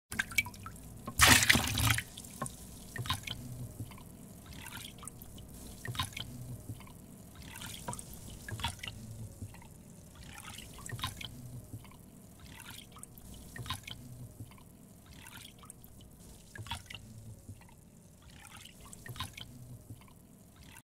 Liquid Test (Blender 2.82)
Just trying out Blender's liquids simulator.